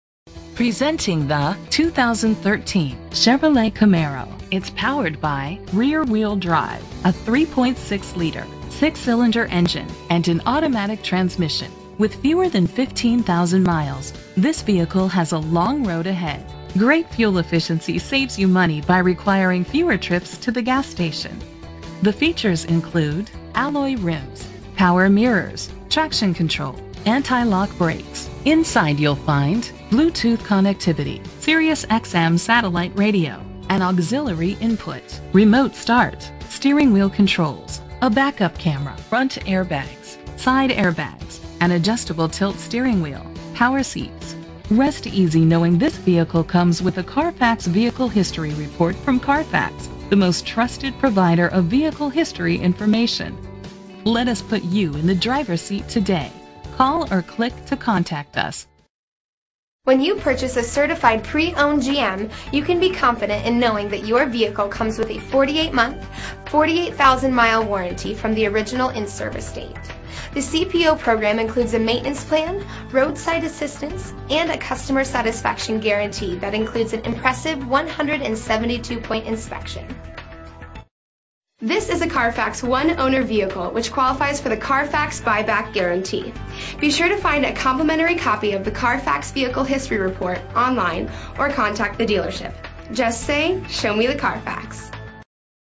2013 Chevrolet Camaro 13098P - Homewood IL
Year: 2013
Make: Chevrolet
Model: Camaro
Trim: LT
Engine: 3.6 liter 6 cylinder RWD
Transmission: Automatic
Color: Red
Mileage: 14405
Address:
18033 Halsted StHomewood, IL 60430
VIN:2G1FB1E32D9131285